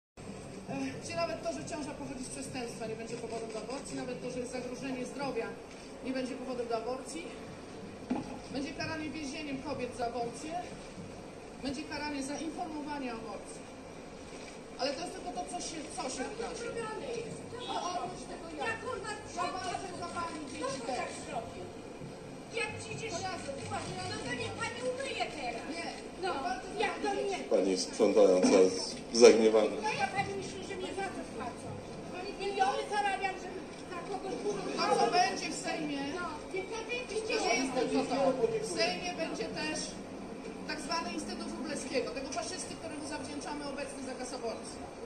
Cleaning lady yells at abortion activist
“I clean this place…Who done this?…Do you think I get paid millions to clean this?” says the cleaning lady. Not sure why Marta Lempart spilled red paint on this door. Thumbnail credit @Artur3891